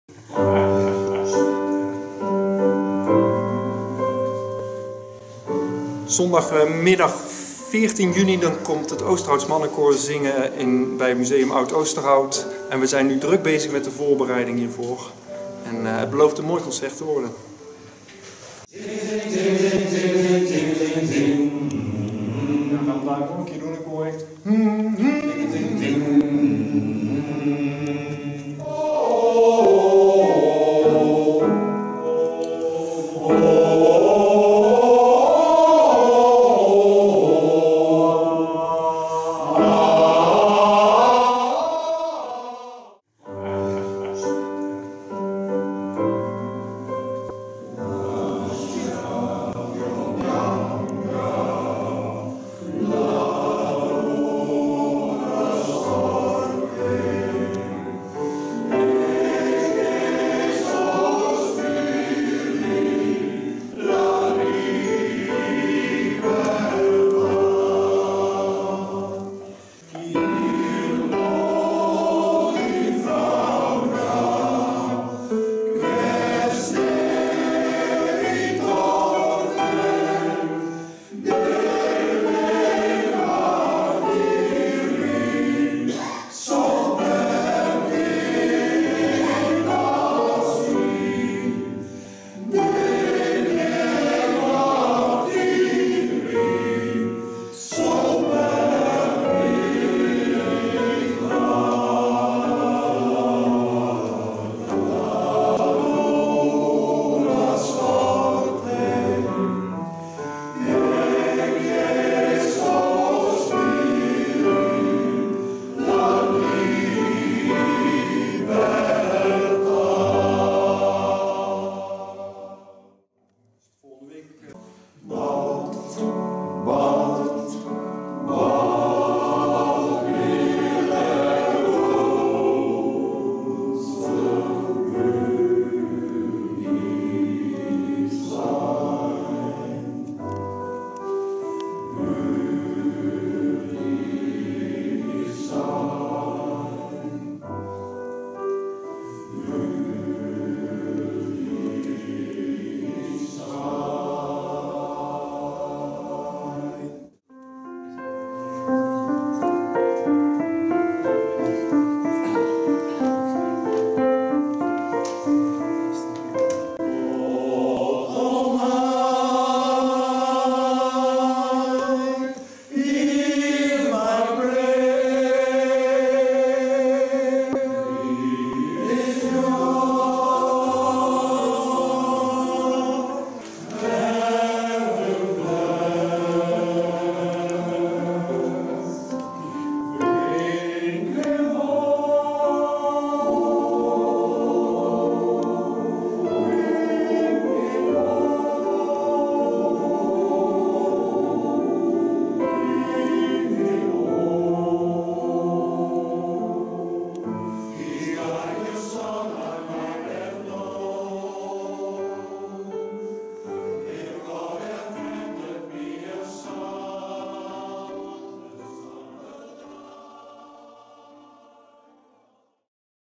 Brabants Museum Oud Oosterhout "Het Oosterhouts Mannenkoor"
Een van de zes koren "Het Oosterhouts Mannenkoor" o.l.v. Heino Vergouwen zingt op zondag 14 juni bij het Brabants Museum Oud Oosterhout. Aanvang 13.40 uur. U bent van harte welkom.